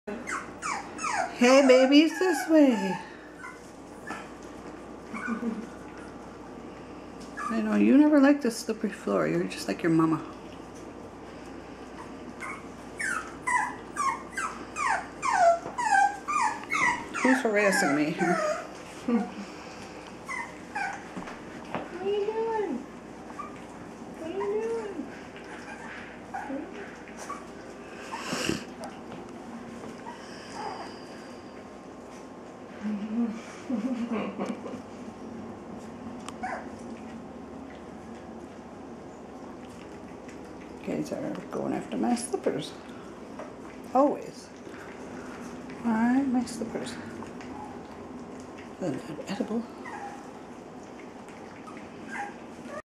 Caballarius King Charles spaniel puppies.
Caballarius Cavalier puppies.  Ch. Brookhaven Heritage Classic x Caballarius Dreamgirl.  Were born Nov 15, 2012 and are almost 6 weeks old in this video.  5 boys and one girl.  The girl is the smallest one.